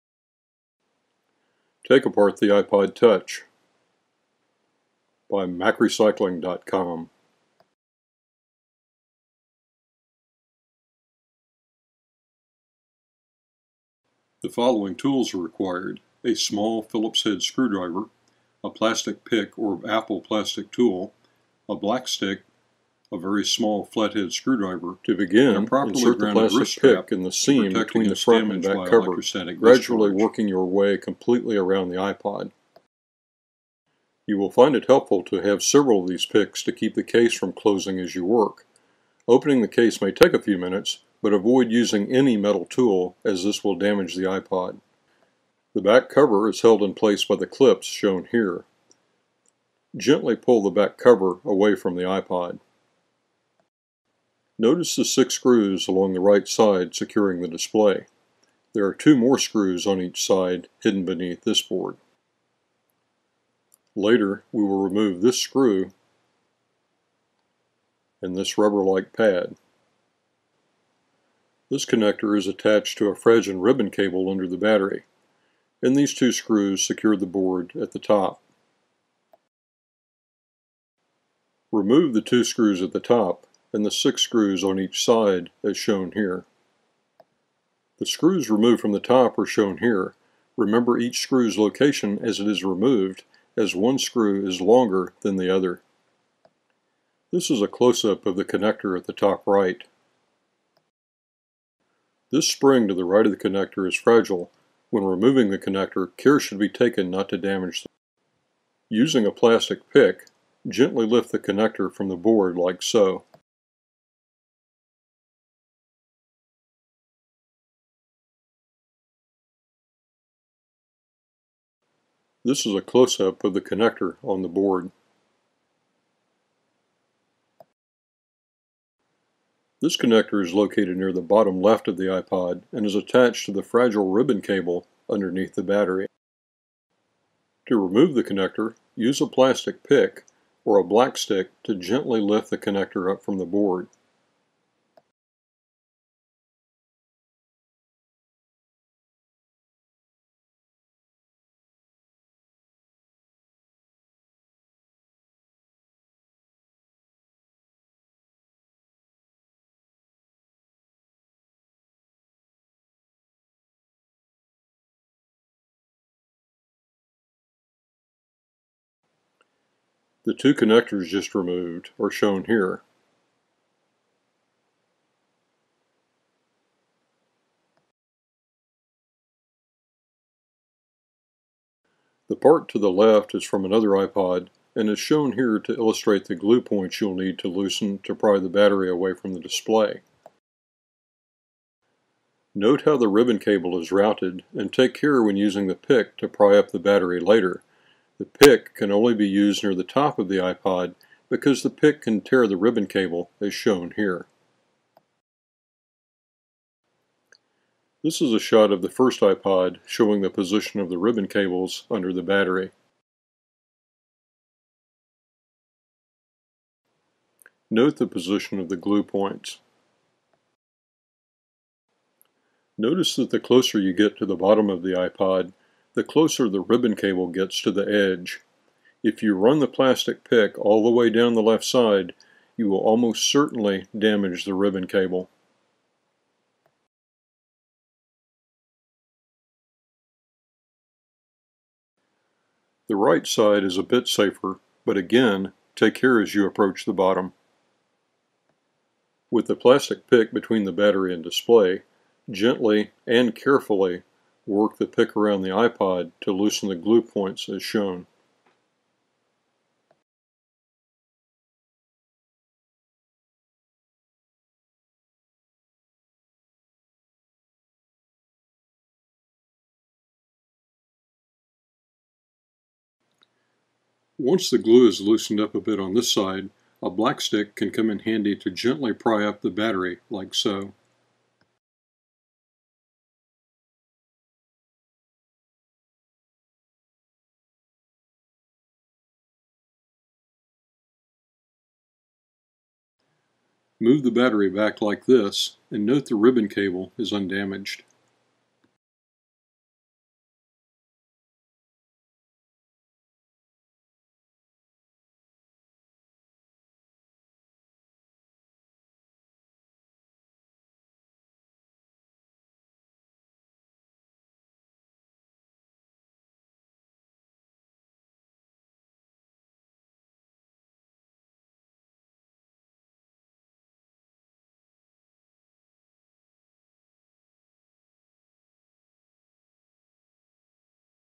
Take Apart Apple iPod Touch 1G - Disassembly Part 1/2
Take Apart Instructions (Guide) for the Apple iPod Touch. Display, battery, logic board, housing.